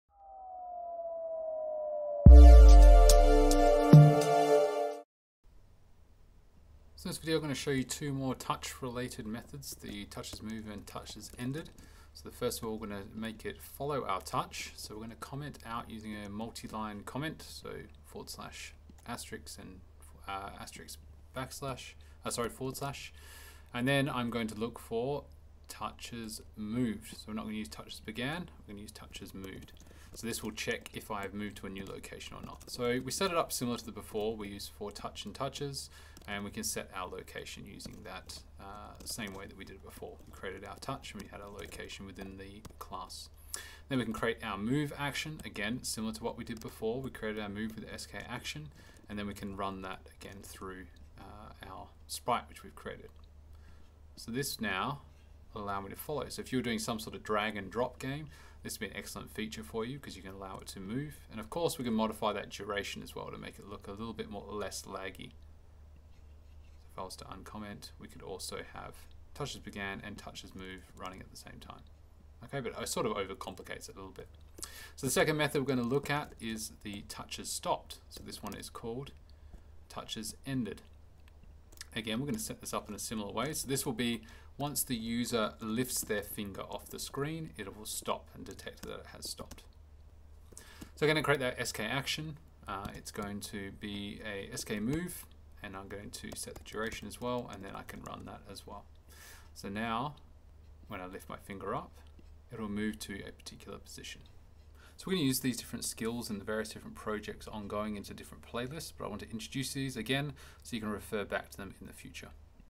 Exploring touchesMoved() and touchesEnded() methods in SpriteKit - Mobile Games Development
This video shows students how to stop use the touchesMoved() and touchesEnded() methods to manipulate Sprite movement in SpriteKit.  This skill will allow learners to create drag and drop style games.